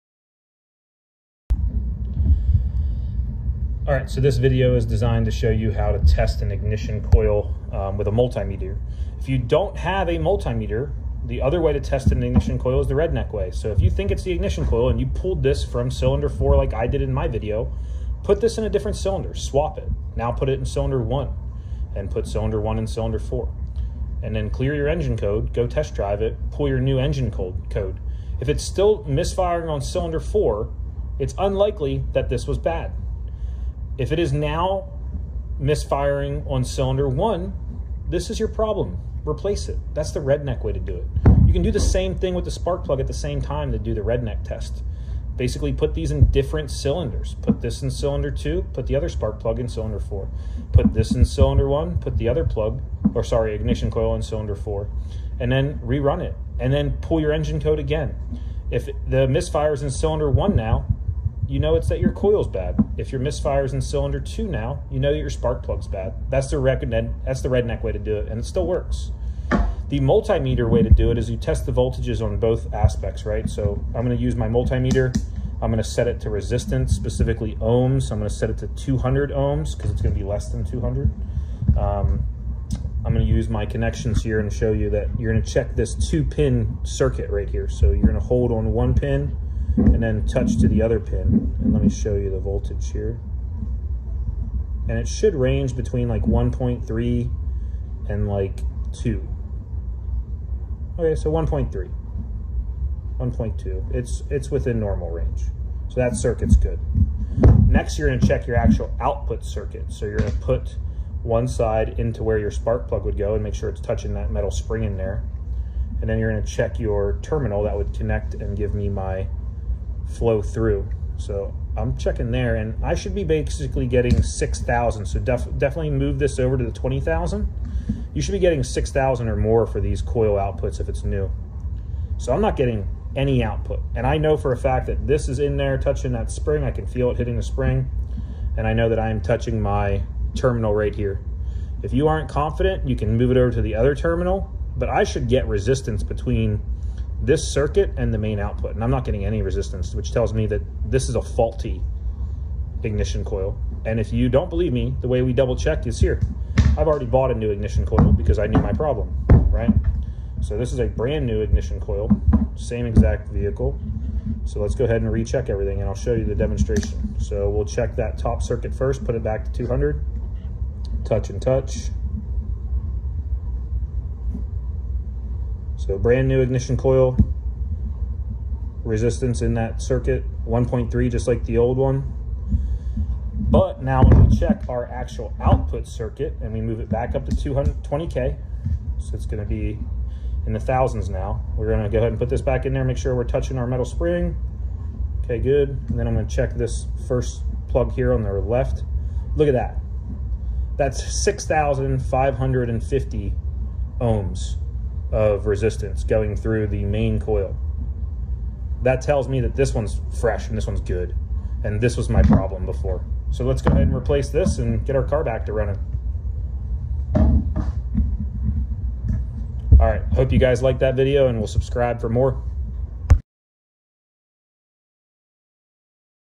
How to test an ignition coil, Diagnose a bad ignition coil. Ignition coil with multimeter, Misfire
This video shows show to diagnose a bad ignition coil with or without a multimeter.

Increased resistance indicates less voltage aka can be going bad.